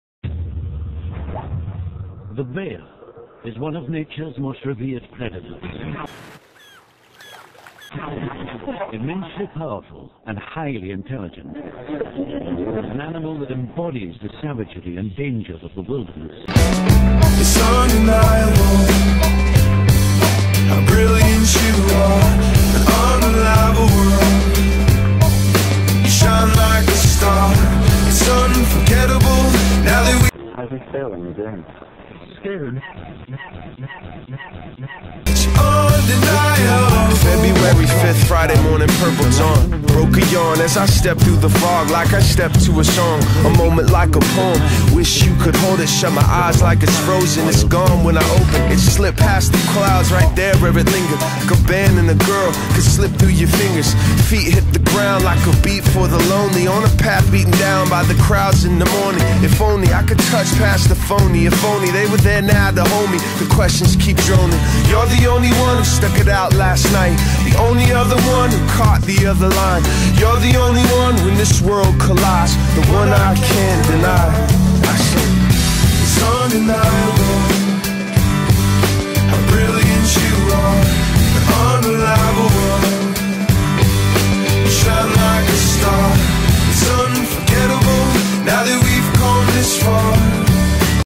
Bear man
Some retard of a man trys to tame bears, Watch him fail again and again!!